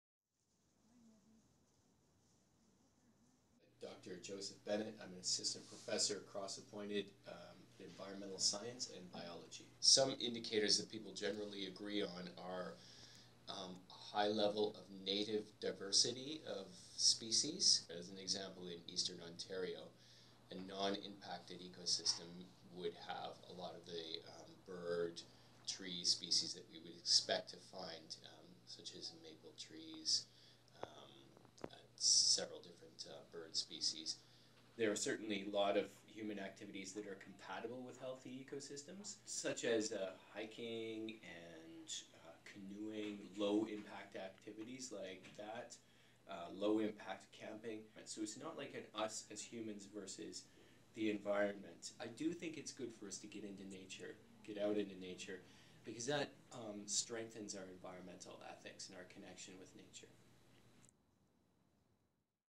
My Video Interview Sequence - Conserving our ecosystem
My interview with Dr. Joseph Bennett about healthy ecosystems and conservation.